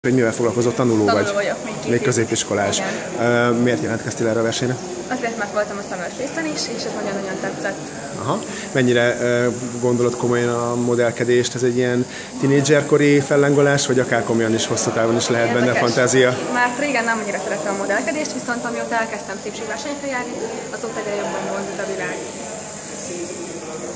Fruzsi, a nap lánya
Fruzsi 17 éves diáklány, aki néhány éve még kézzel-lábbal tiltakozott minden ellen, ami modellkedés vagy szépségverseny, ám egy éve belekóstolt ebbe a világba, amelyben azóta nagyon jól érzi magát. Fotó: Racskó Tibor